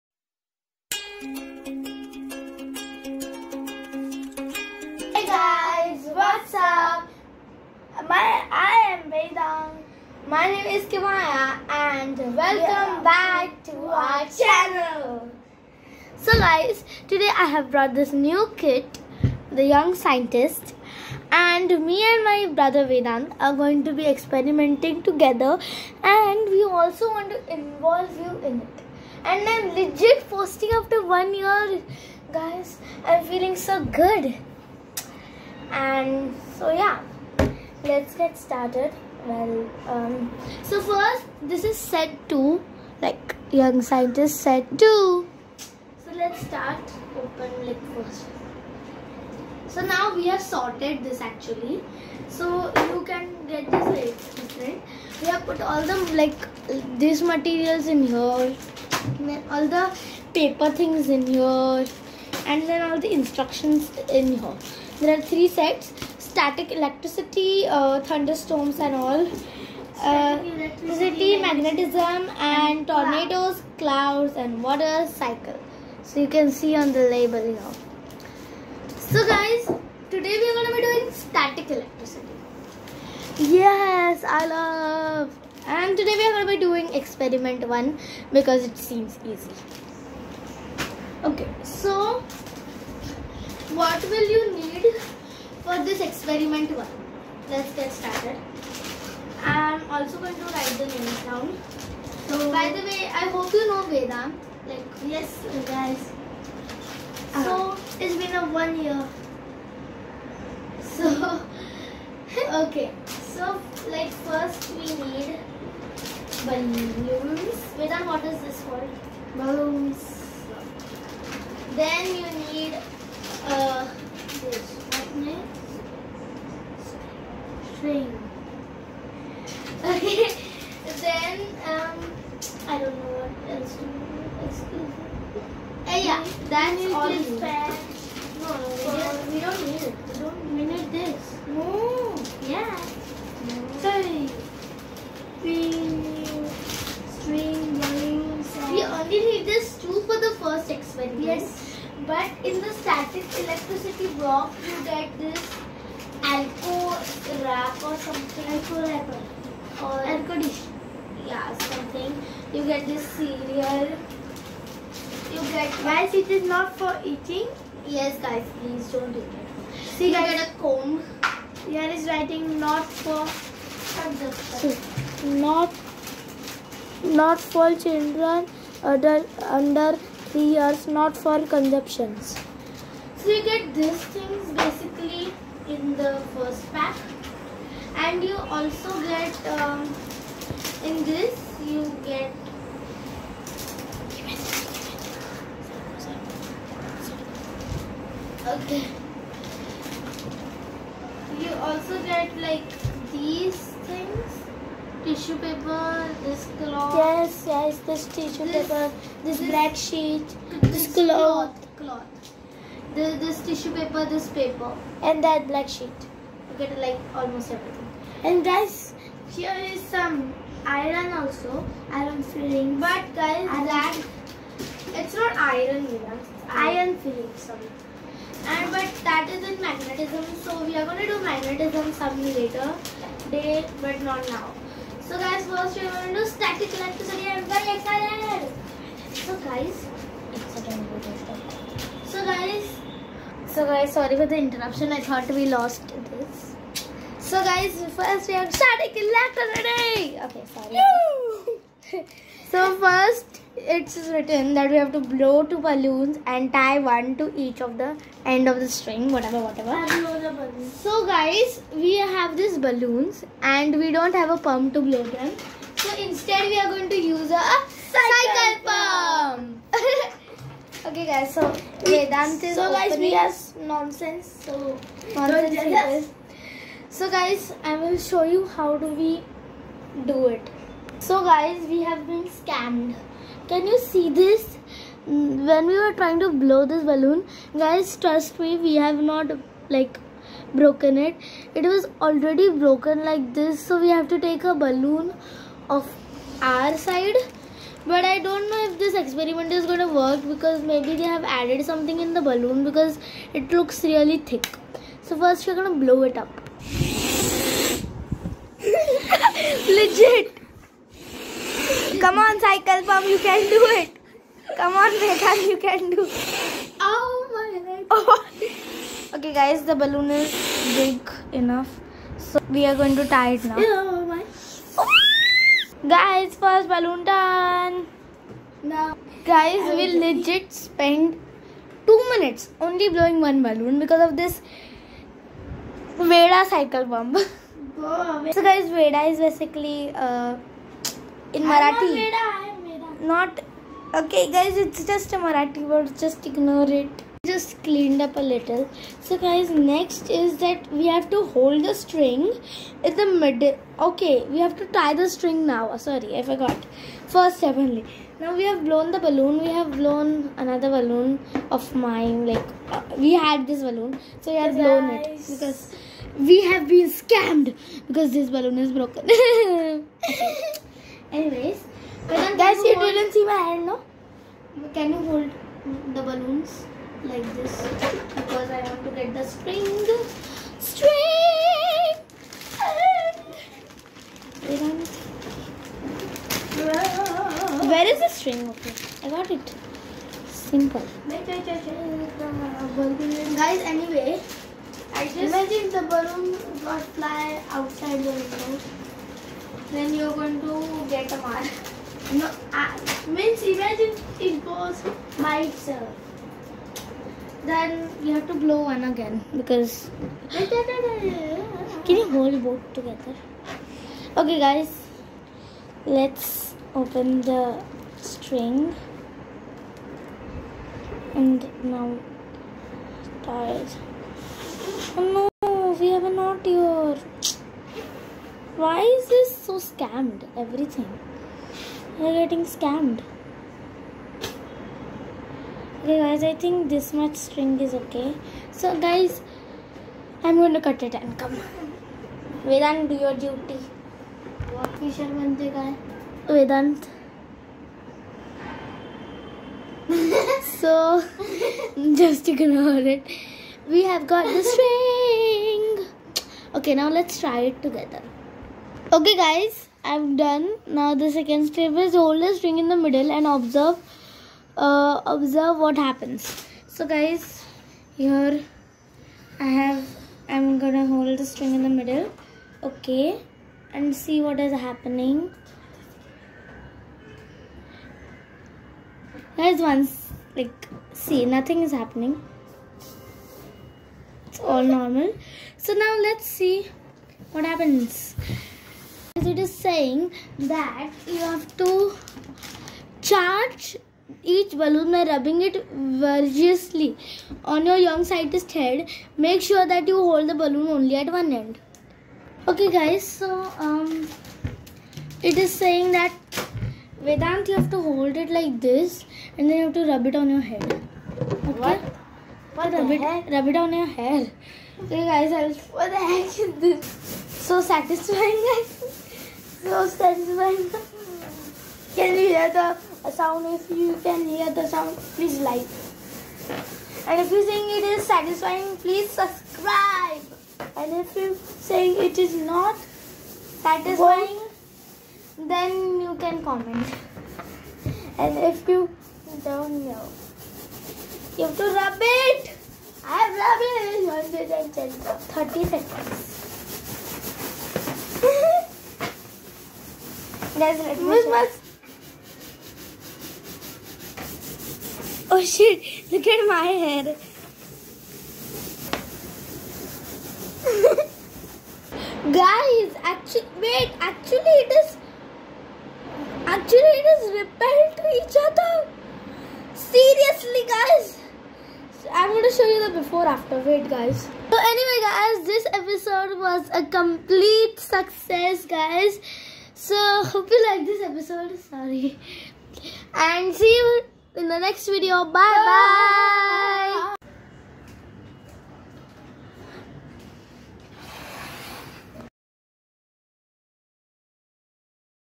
How to use the young scientist set 2|Static electricity experiment 1|vlog with my brother
here we have brought the young scientist set 2 and we have done static electricity experiment one..the video is fully detailed on how to do it